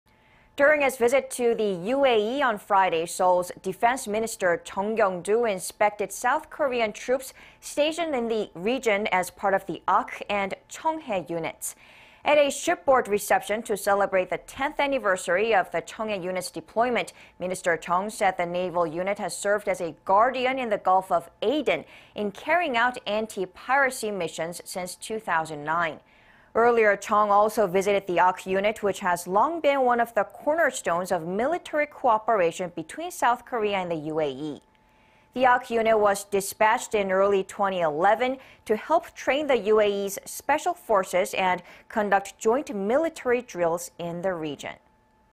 Defense Minister Jeong visits Ahk and Cheonghae units in UAE
정경두 국방부 장관, 청해•아크부대 장병 격려
During his visit to the UAE on Friday,... Seoul's Defense Minister Jeong Kyeong-doo inspected South Korean troops stationed in the region as part of the Ahk and Cheonghae units.
At a shipboard reception to celebrate the 10th anniverysary of the Cheonghae unit's deployment,... Minister Jeong said the naval unit has served as a guardian in the Gulf of Aden... in carrying out anti-piracry missions since 2009.
Earlier, Jeong also visited the Ahk unit... which has long been one of the cornerstones of military cooperation between South Korea and the UAE.
The Ahk unit was dispatched in early 2011... to help train the UAE's special forces and conduct joint-military drills in the region.